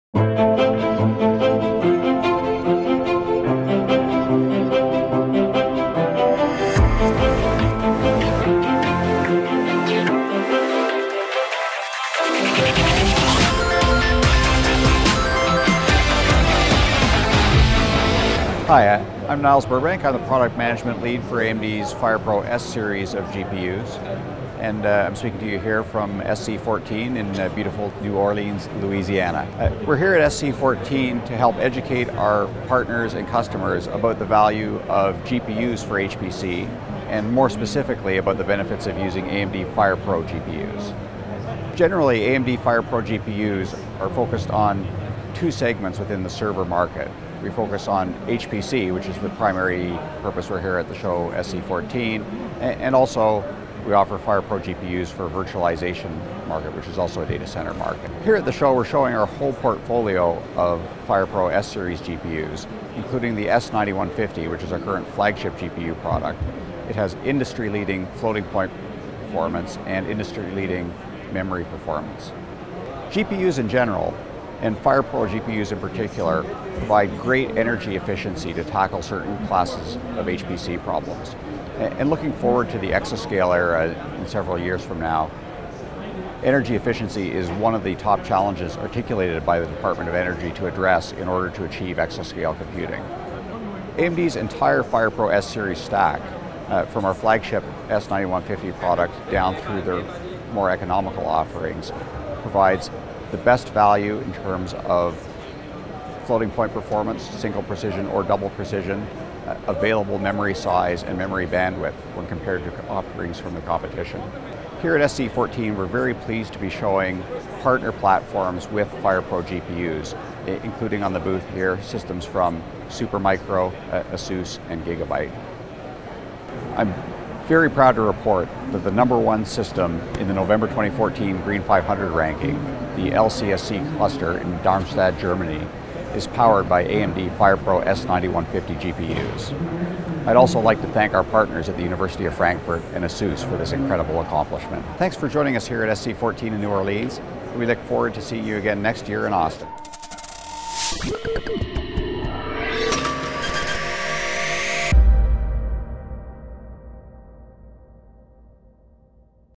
AMD @ SC14
Niles Burbank talks about AMD FirePro solutions at SC14 in New Orleans.